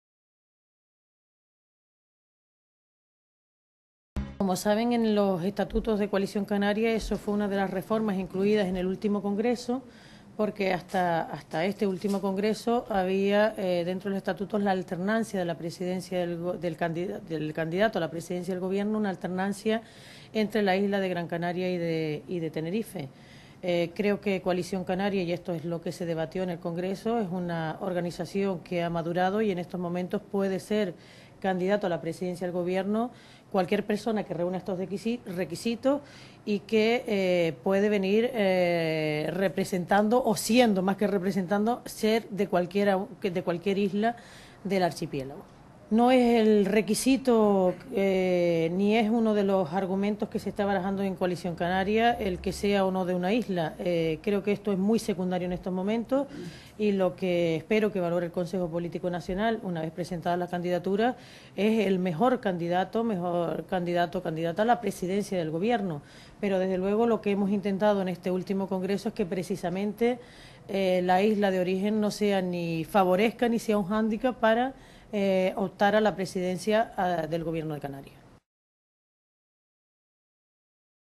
Claudina Morales habla de la alternancia en las candidaturas
La presidenta de Coalición Canaria, Claudina Morales, habla de la no alternancia en las candidaturas a la Presidencia del Gobierno